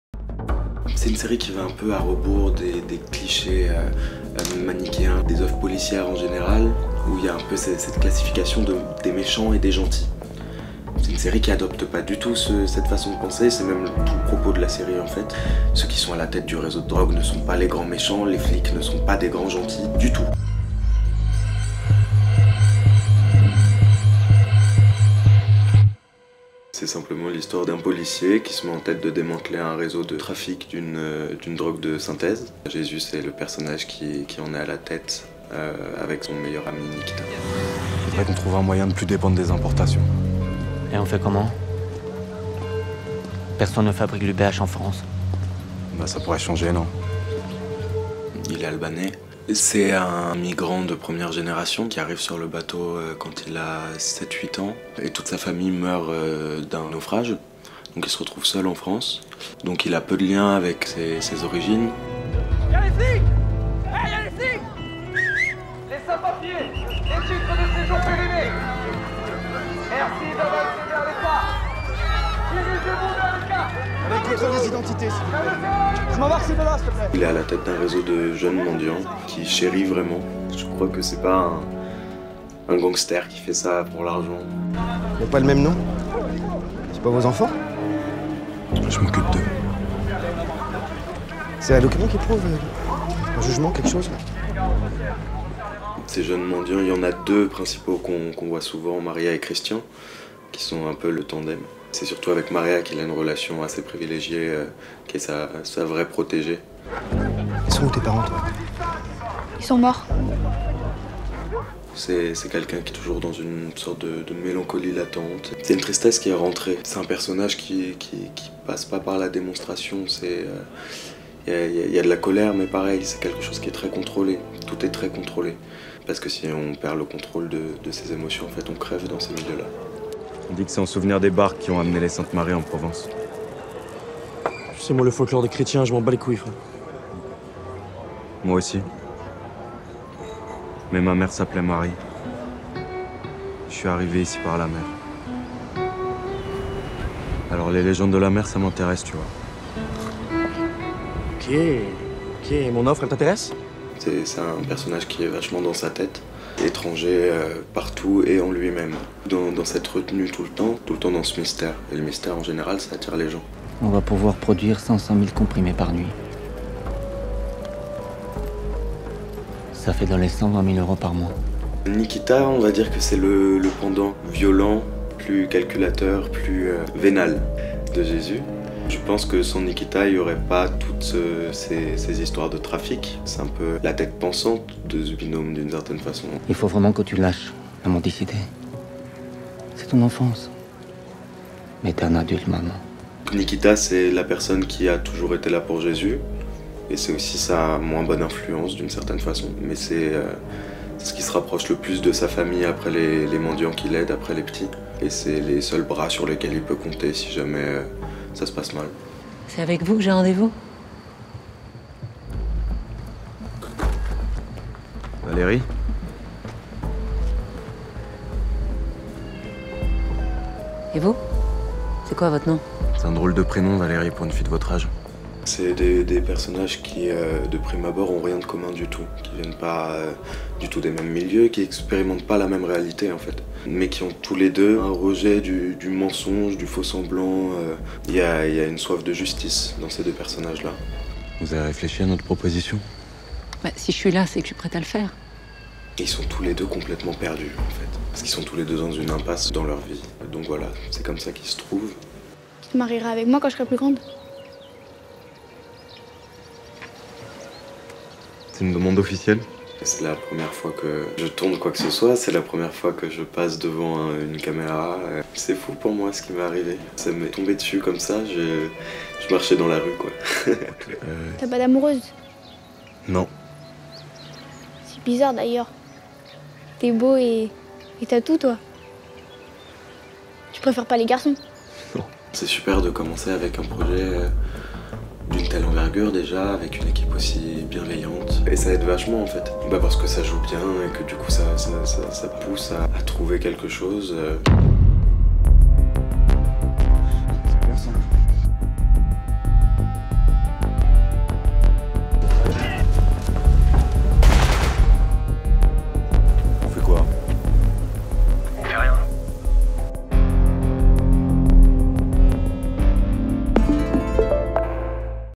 [Interview] Infiltré(e) : Sumaï Cardenas
Chimiste dans la police, Aurélie élève seule son fils depuis la mort de son compagnon. Quand le commissaire Max Vernet a recours à ses compétences pour enquêter sur l’UBH, une nouvelle drogue de synthèse, qui commence à faire des victimes, elle ne se doute pas qu’elle va se retrouver infiltrée au cœur d’un réseau de jeunes et dangereux trafiquants. Face au charismatique Jésus, qui tient le réseau depuis Marseille, a-t-elle une chance de mener à bien sa mission ? Pour sauver son fils, Aurélie n’a plus le choix.

Série (2023 – inédite) – Durée 6 x 52 min – Réalisation – Jean-Philippe Amar – Créée par Frédéric Krivine & Emmanuel Daucé – Écrite par Frédéric Krivine –Production Tetra Media Fiction Avec la participation de France Télévisions
Avec : Audrey Fleurot (Aurélie/Valérie), Thierry Neuvic (Max), Sumaï Cardenas (Jesus), Stéphane Soo Mongo (Kerjean), Bogdan Zamfir (Nikita), François Loriquet (Picard)...

À partir du lundi 25 septembre sur France 2